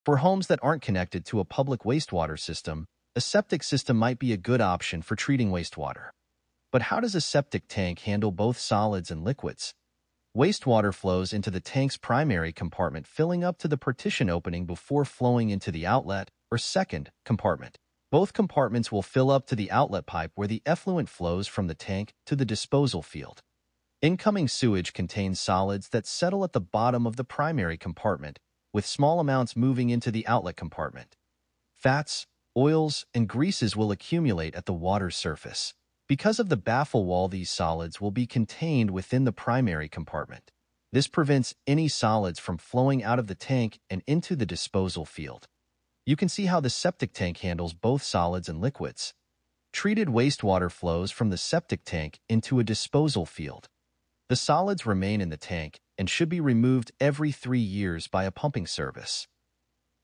How Does A Septic Tank Handle Liquids AND Solids?
For homes not connected to a public wastewater system, a septic tank may be a good option for treating wastewater. But how does a septic tank handle both solid and liquid waste?